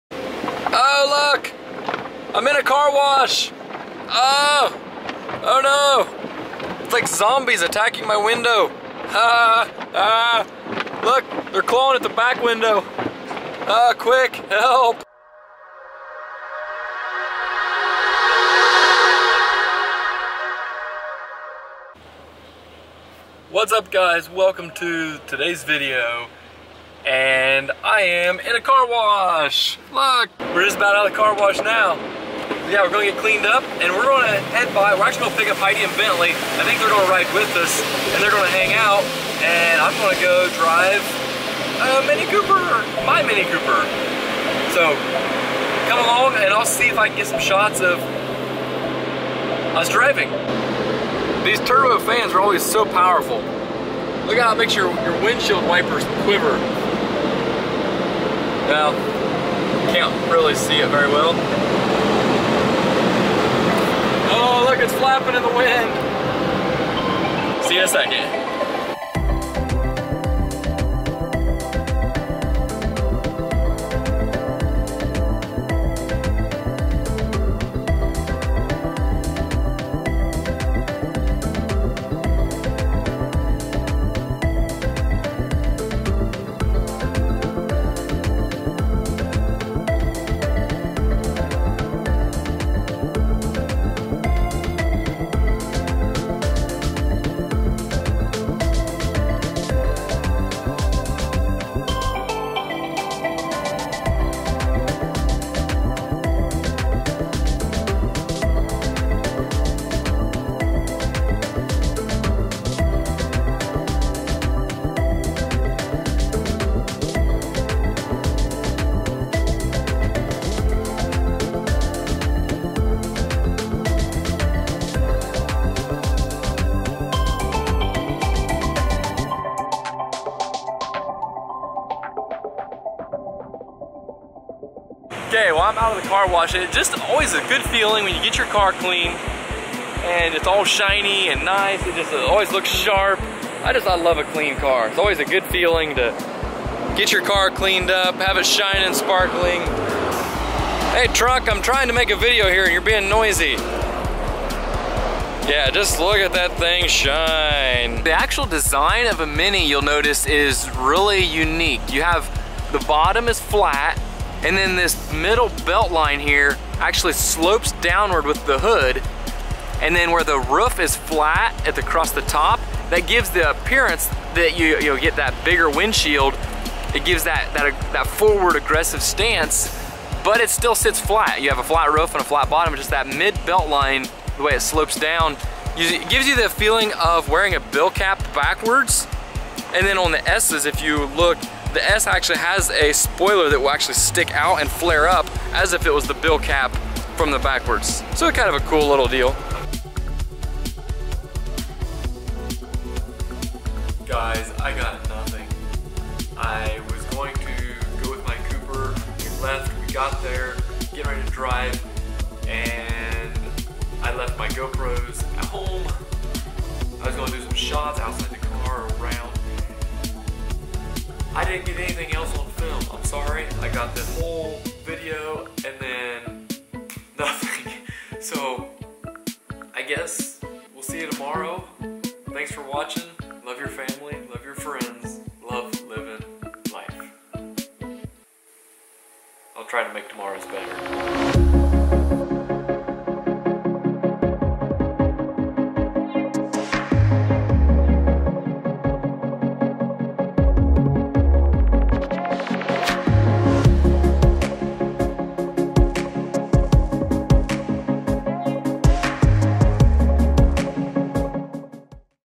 Love a Clean Car
Day 25 of my 30 day video challenge. Spencer and I are each posting a video a day for 30 days.

Got my MINI Cooper all cleaned up and bathed.

I'd love some feedback on ways to improve or video ideas. Thanks!